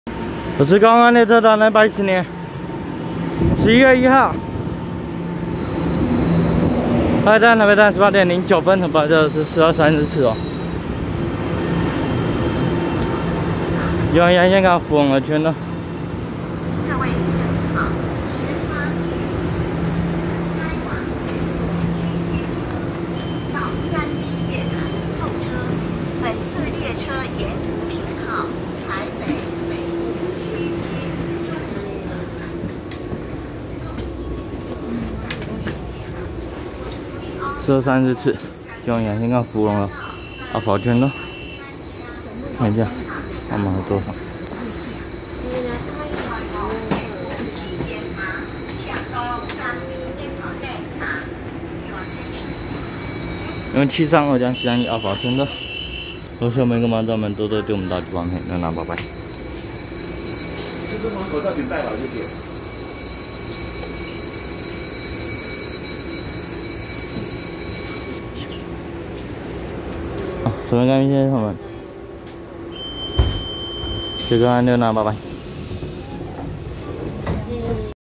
4234次北湖發車經由宜蘭線開往福隆的EMU732+EMU731阿福號區間車
110/11/1台北站18:09分拍攝4234次北湖發車經由宜蘭線開往福隆的EMU732+EMU731阿福號區間車